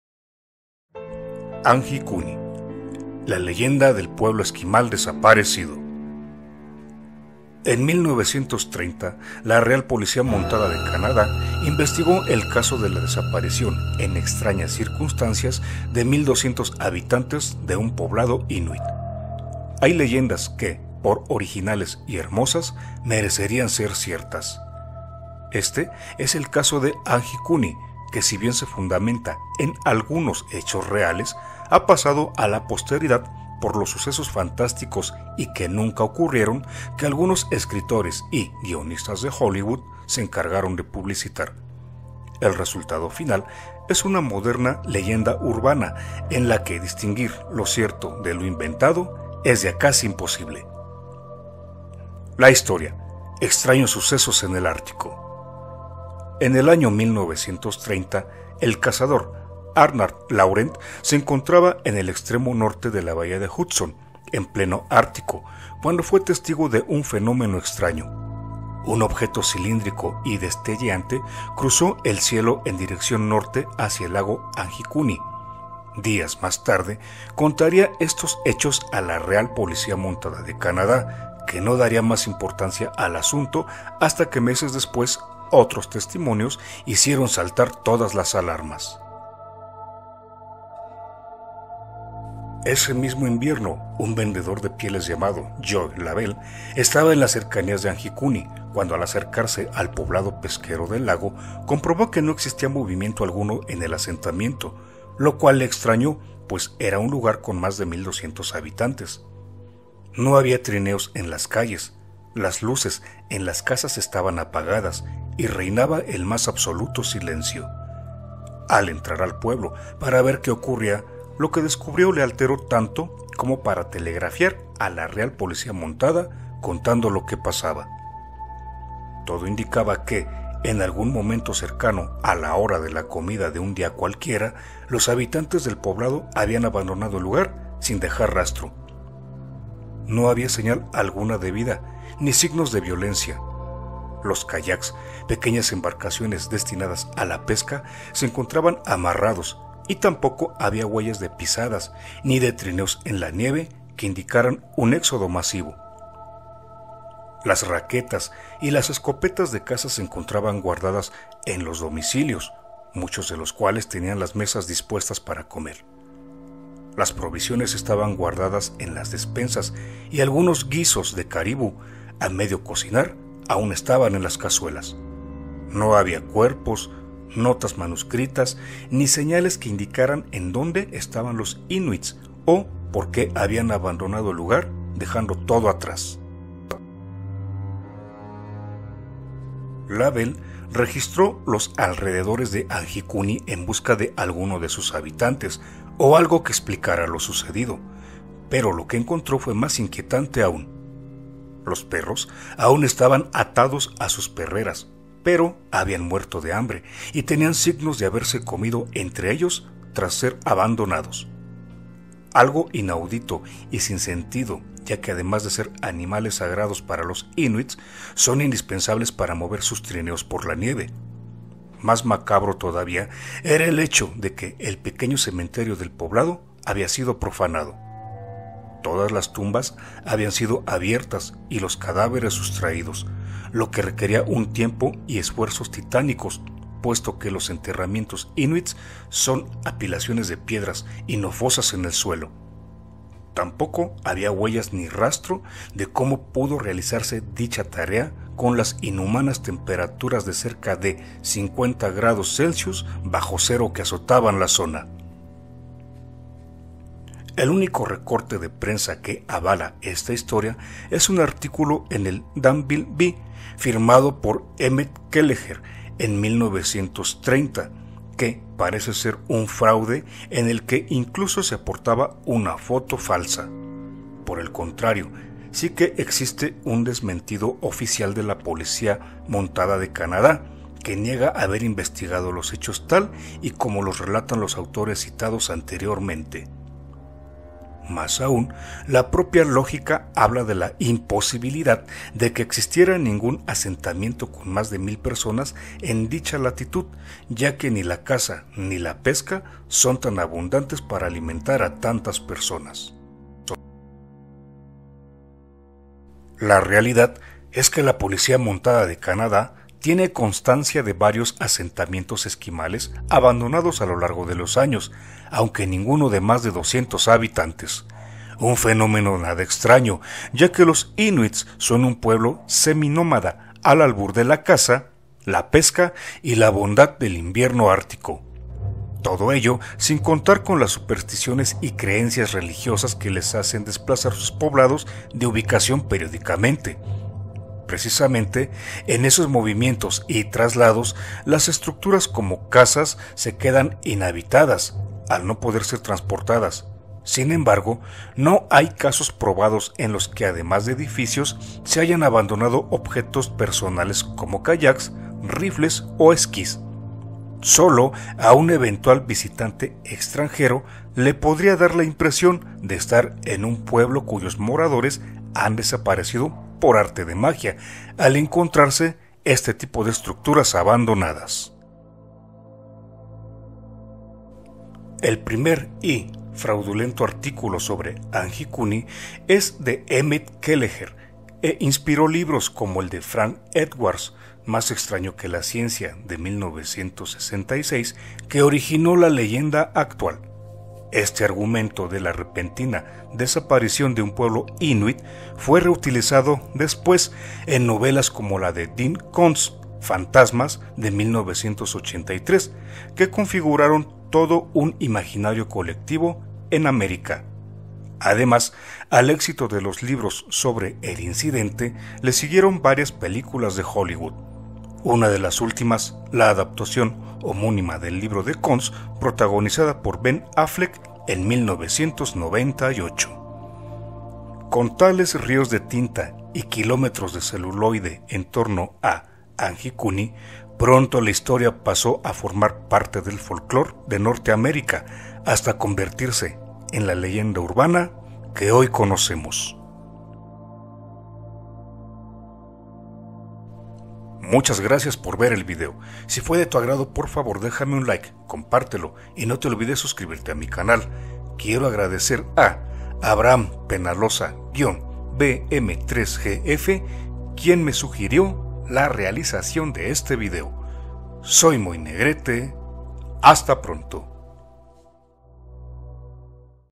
ANJIKUNI, EL PUEBLO ESQUIMAL DESAPARECIDO
A principios de la década de 1930, un poblado inuit que habitaba a las orillas del lago Anjikuni en Canadá, desapareció misteriosamente sin dejar rastro alguno. Esta es una leyenda urbana que ha pasado a formar parte del folclor canadiense y que ha inspirado a la realización de libros y películas desde hace varios años.
Fuente de la información: Internet.